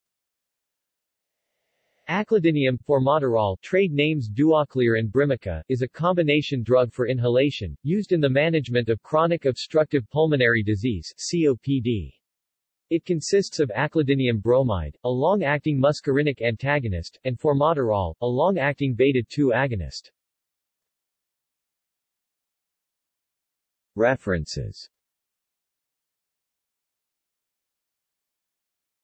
Aclidinium/formoterol | Wikipedia audio article
This is an audio version of the Wikipedia Article:
Aclidinium/formoterol

SUMMARY
Aclidinium/formoterol (trade names Duaklir and Brimica) is a combination drug for inhalation, used in the management of chronic obstructive pulmonary disease (COPD). It consists of aclidinium bromide, a long-acting muscarinic antagonist, and formoterol, a long-acting β2 agonist.